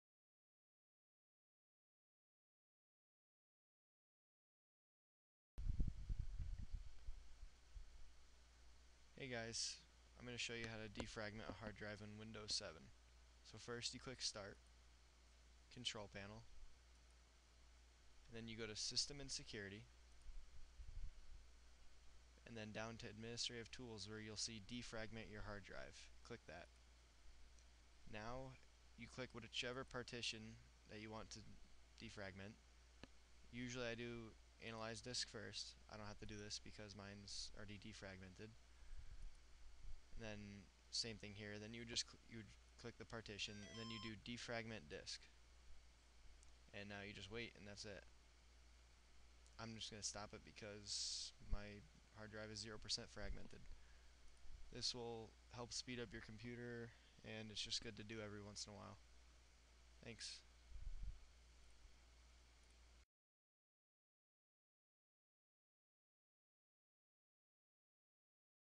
How to defragment a HardDrive in Windows 7 (HD)
(Watch in HD!)   This is my first tutorial where I will show you how to defragment a harddrive in windows 7. This is a very basic beginner level tutorial. I will make atleast one tutorial per day if all goes well. I apologise if my voice is quiet, my microphone is getting old and needs to be replaced.